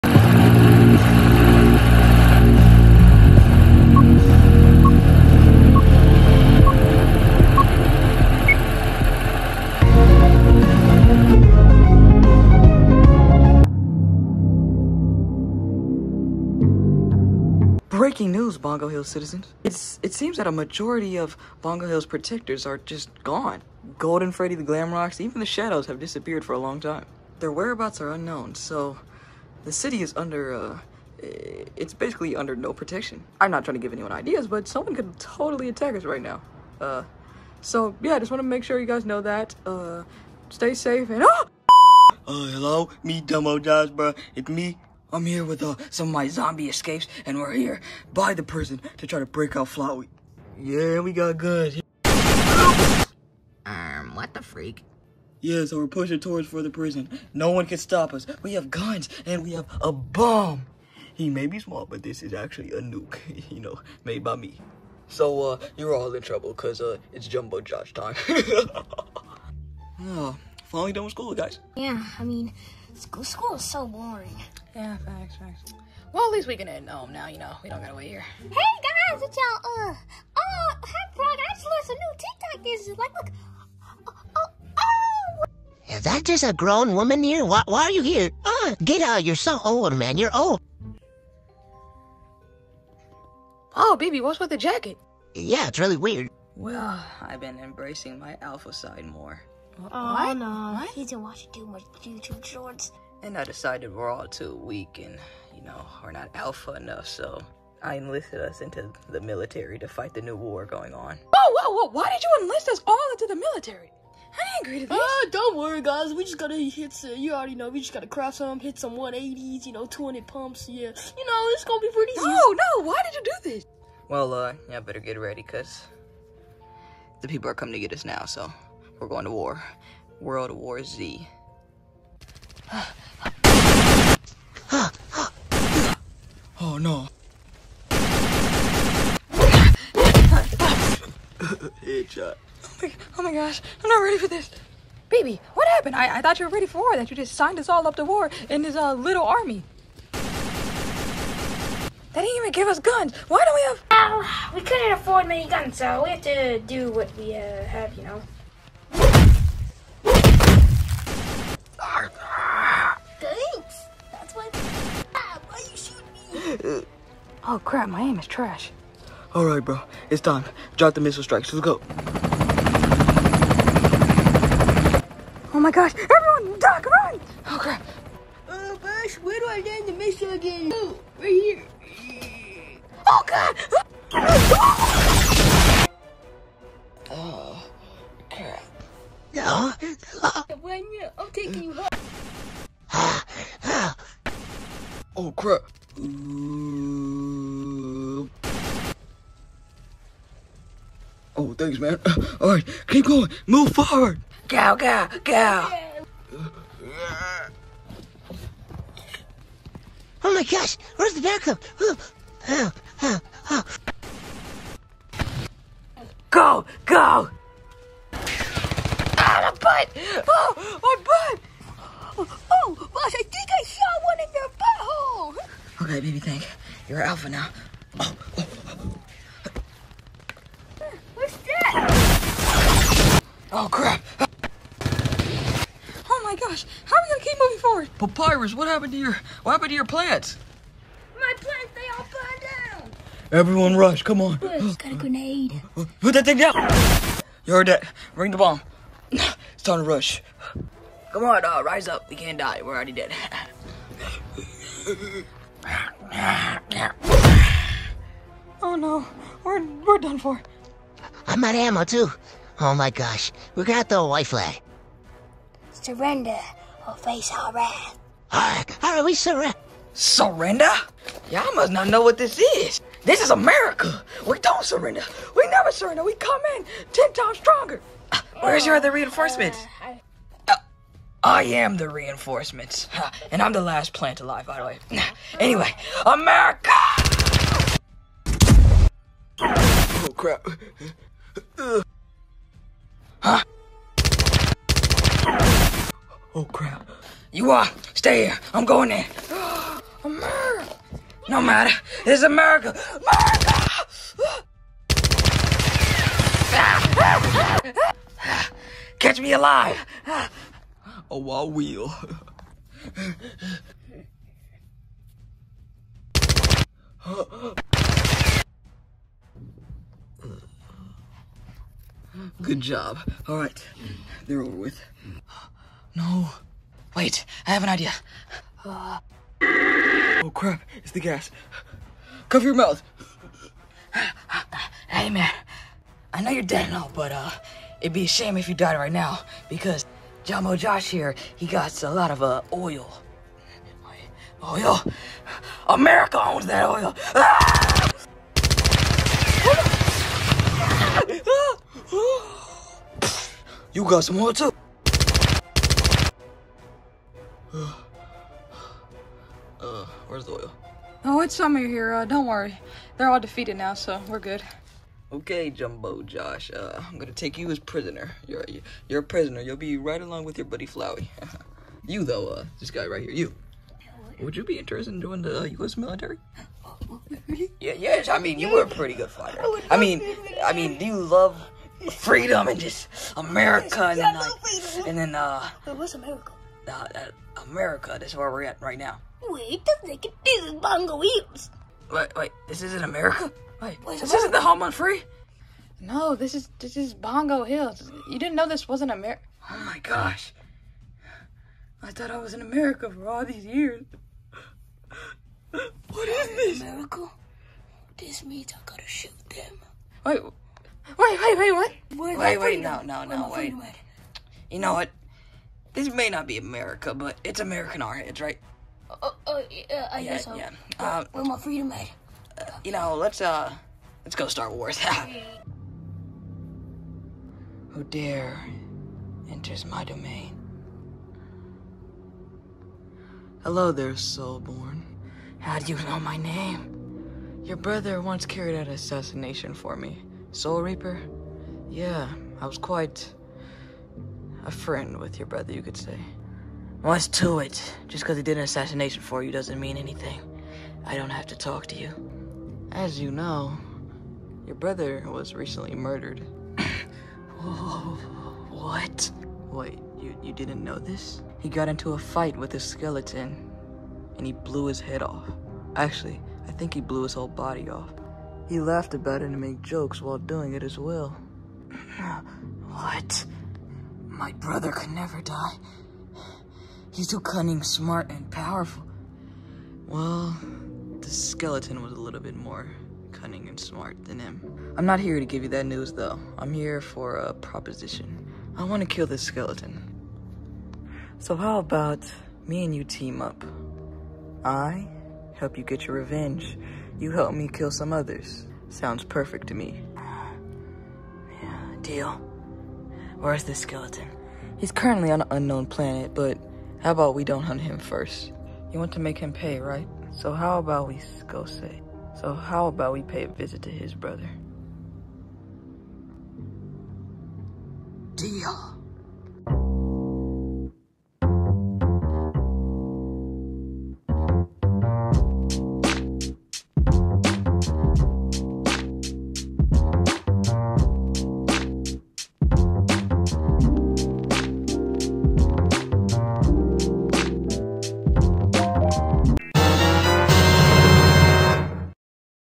Fnaf plush: world war z
After the mysterious disappearance of bongo hills hero’s jumbo Josh and his zombie army decides to try to free there leader flowey and stars a war ‘world war Z’